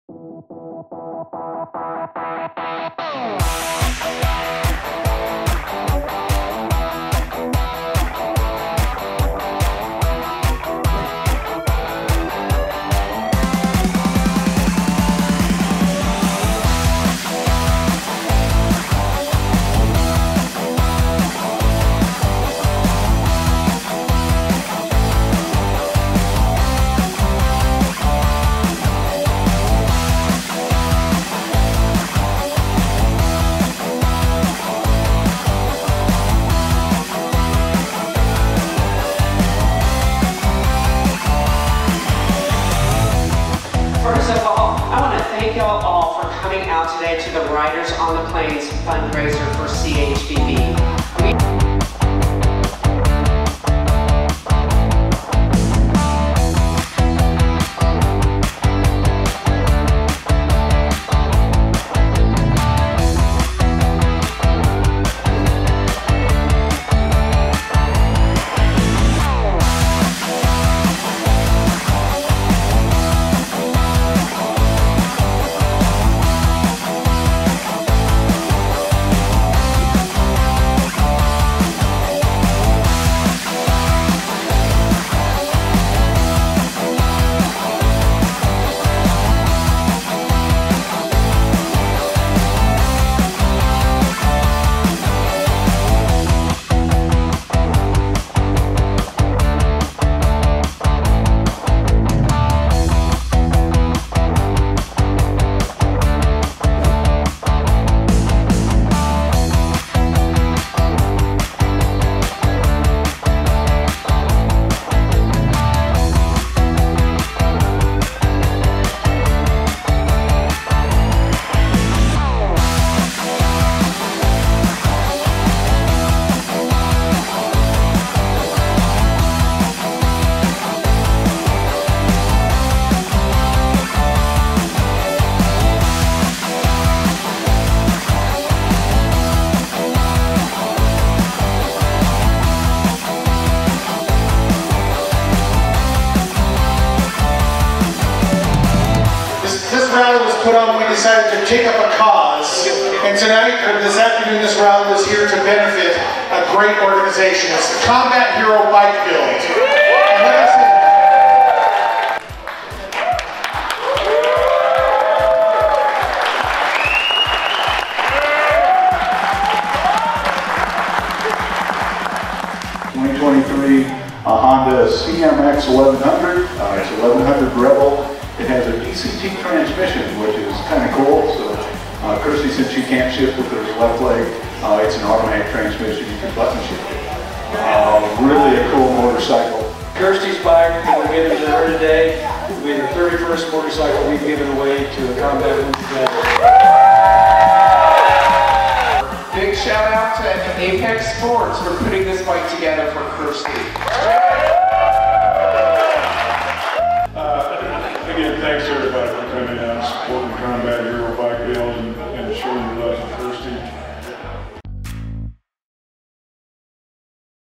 2023 Riders on the Plains Poker Run to benefit the Combat Hero Bike Build
We were honored to capture the 2023 Riders on the Plains Poker Run from Watkins to Byers, Colorado to benefit Combat Hero Bike Build as they presented their 31st custom-built motorcycle to Kirstie Ennis. Learn more about this wonderful organization and the work they do at CombatHeroBikeBuild.org.